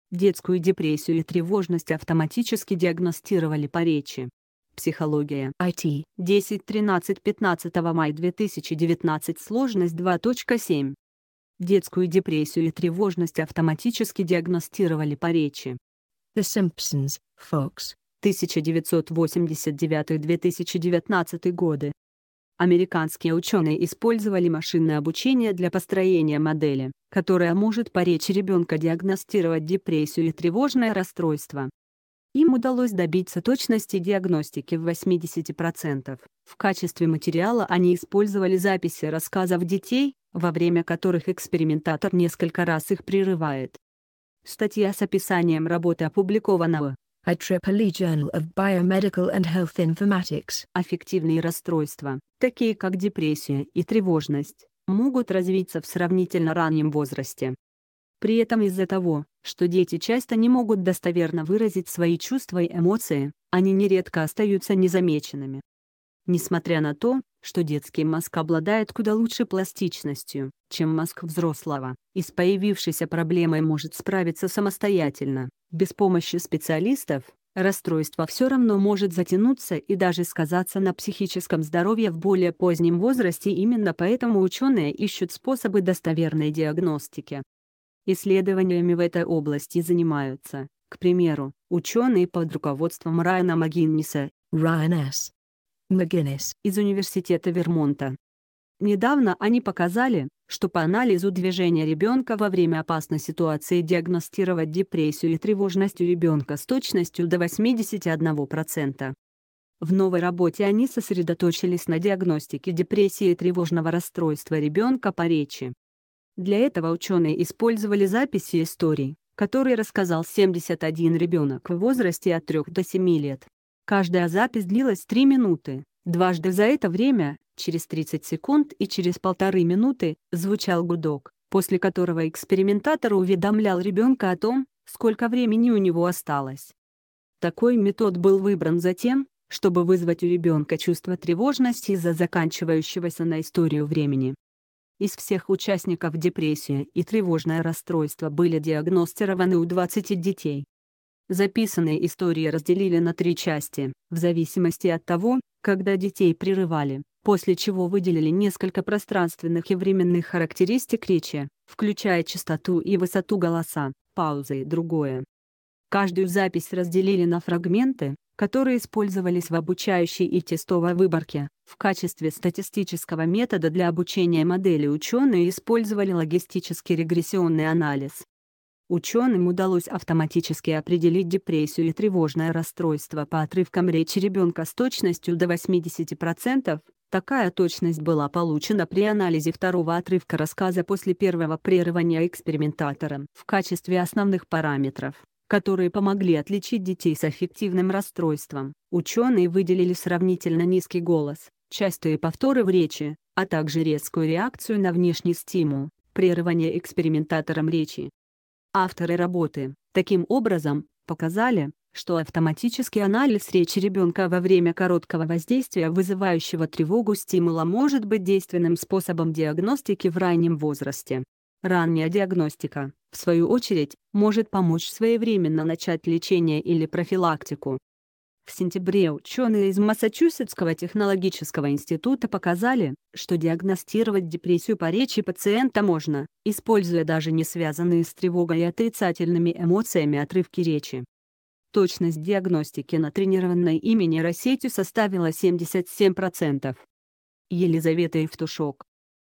Детскую депрессию и тревожность автоматически диагностировали по речи
Детскую депрессию и тревожность автоматически диагностировали по речи Психология IT 10 13 15 Май 2019 Сложность 2 .7 Детскую депрессию и тревожность автоматически диагностировали по речи The Simpsons FOX, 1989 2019 Американские ученые использовали машинное обучение для построения модели, которая может по речи ребенка диагностировать депрессию и тревожное расстройство . Им удалось добиться точности диагностики в 80 процентов в качестве материала они использовали записи рассказов детей